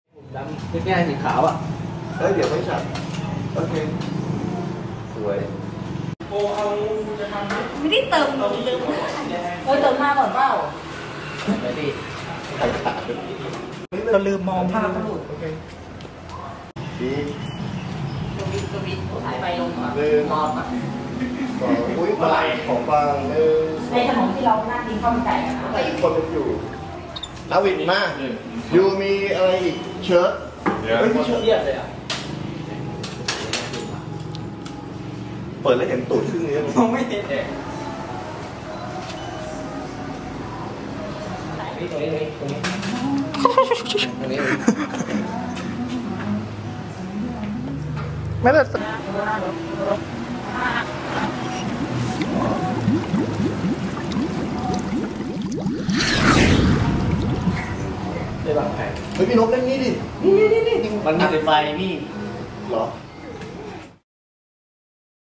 Live สด นางแบบ hot model in the pool
เมื่อเราพา นางแบบ มาไลฟ์สด
ริมสระว่ายน้ำ จะแซ่บขนาดไหน
ตามมาดูกันเลย